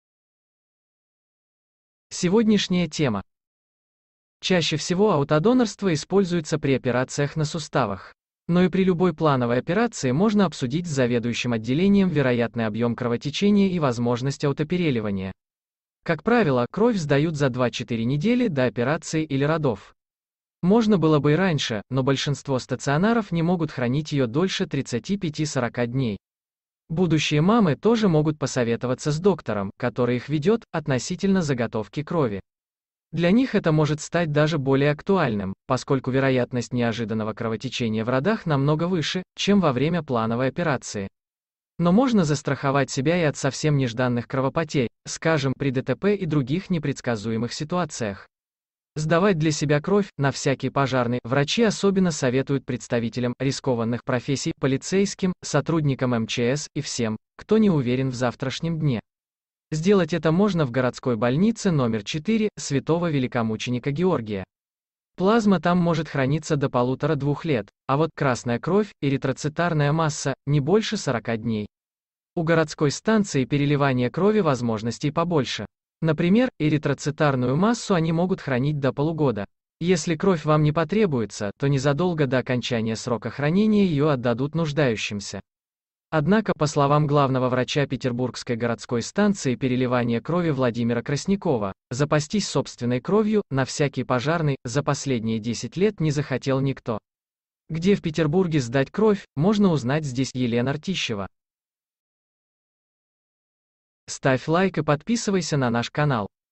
Петербуржцы могут заготавливать кровь впрок, но не хотят
У петербуржцев есть возможность застраховать себя на случай потери кровиnbsp; во время операции или родов и сдать кровь для самих себя впрок. Доноров в городе не хватает, поэтому для тех, кто знает, что им понадобится кровь, аутодонорство очень удобно. Особенно хорош этот вариант для людей с редкими группами крови.